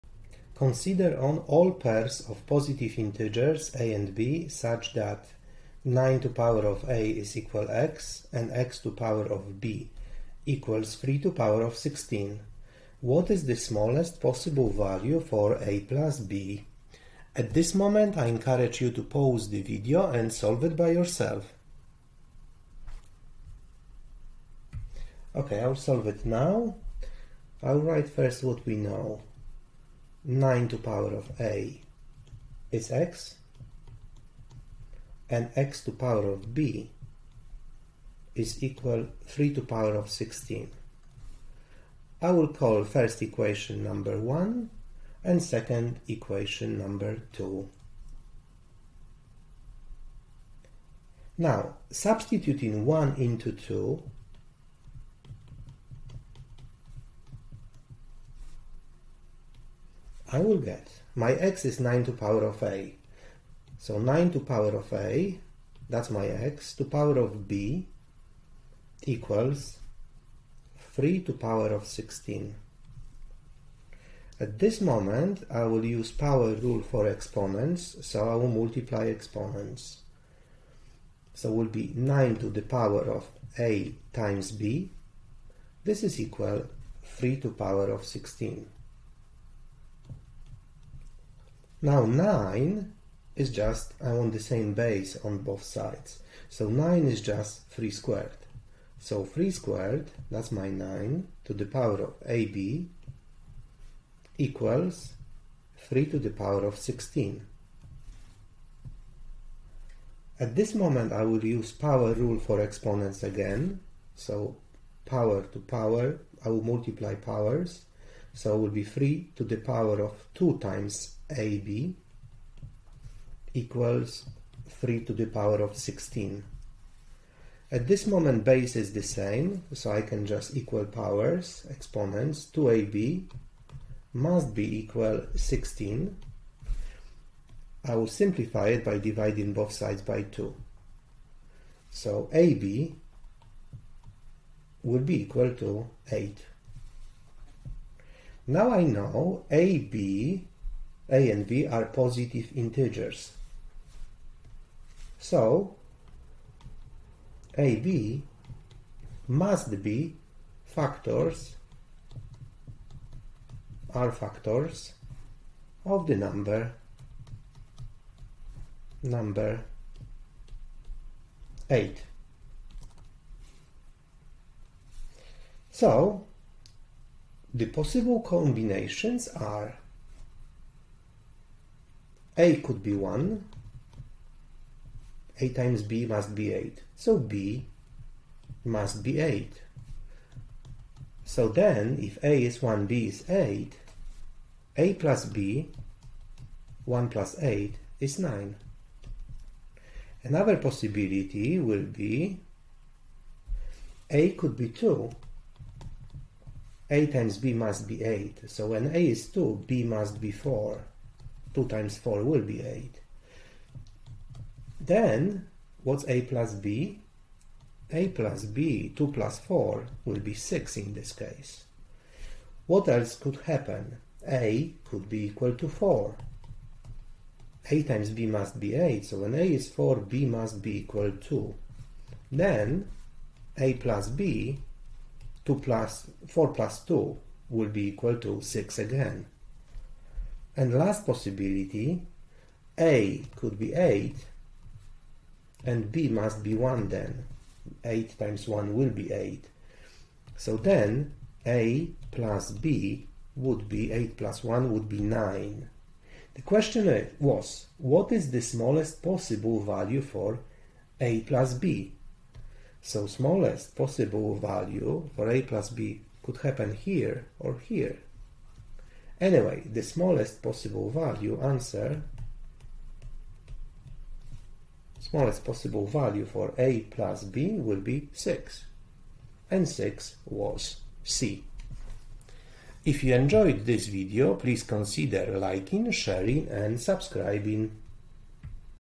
ACT Hard Problem 2 (Algebra Problem)
Consider all pairs of positive
integers  a  and  b  such that:
9^a=x   and   x^b=3^16.
What is the smallest possible value
for    a+b ?

A) 4
B) 5
C) 6
D) 8
E) 9